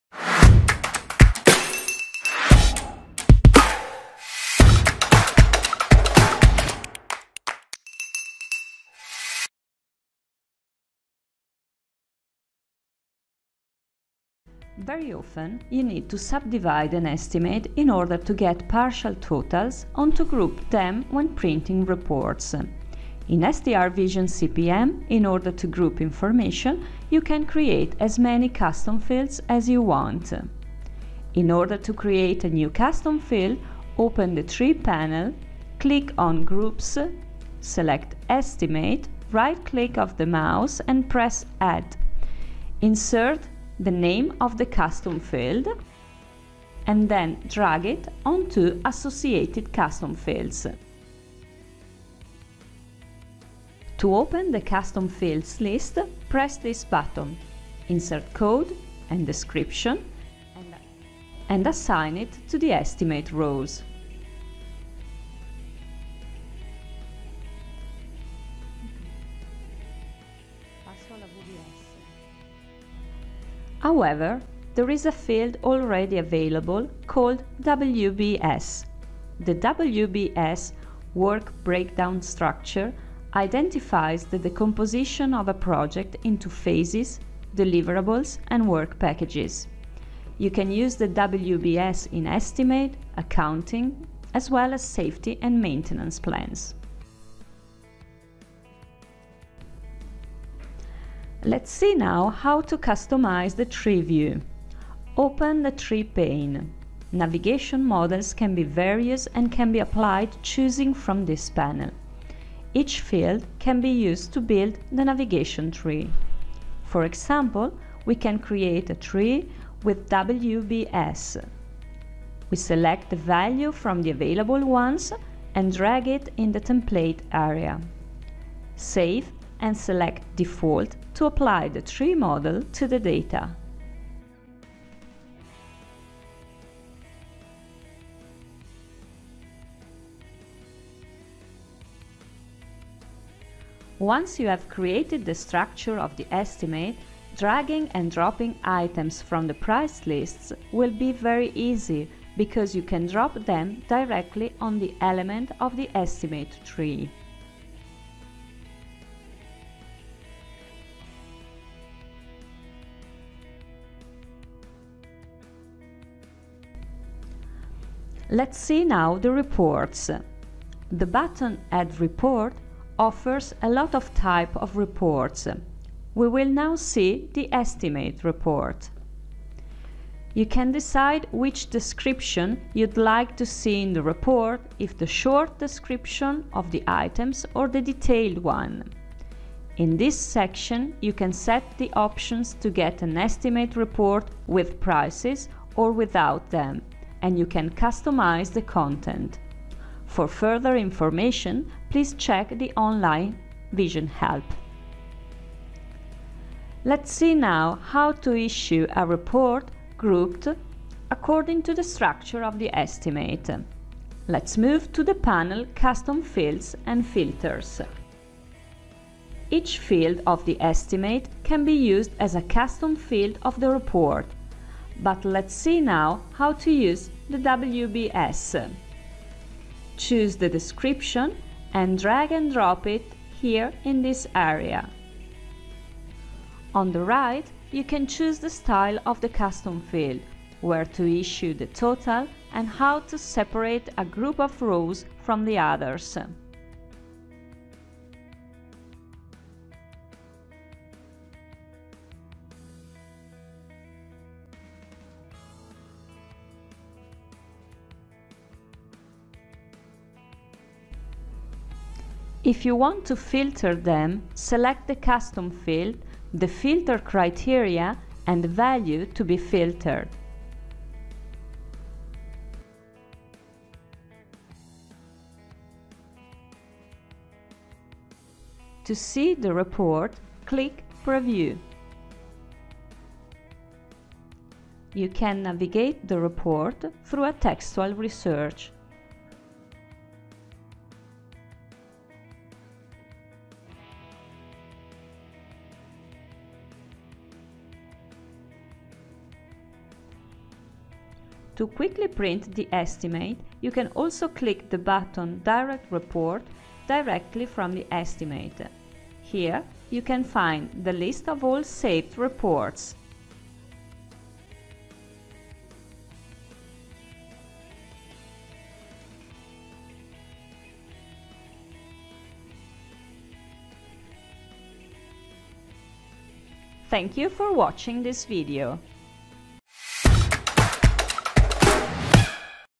Budgeting & Planning Estimate Part 2 | STR Vision CPM Tutorial
Creating groups and managing the Navigation tree, using WBS and issuing reports